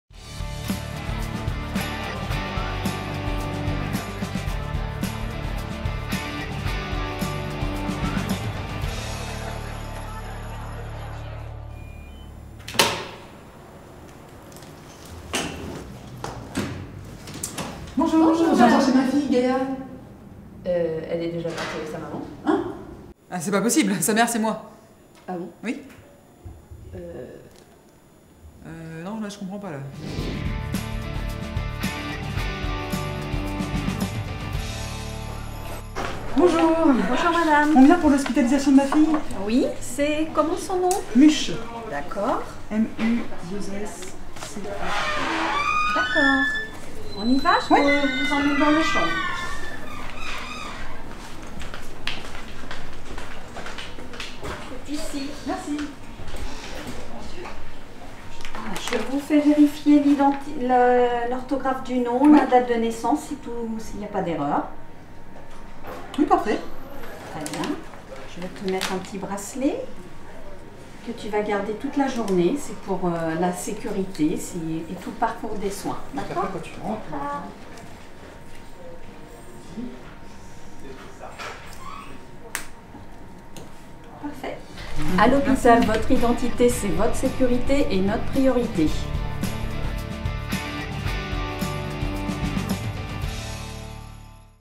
Identito-Vigilance : Episode 1/4
Découvrez à travers une série de 4 épisodes, comment le CHU sécurise votre prise en charge grâce à votre identité.